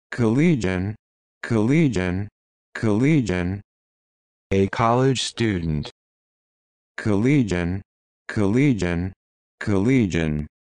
SAT Vocabulary Words and Definitions — Collegian
This video shows you how to say or pronounce Collegian and gives the definition of Collegian.

What's your definition of Collegian?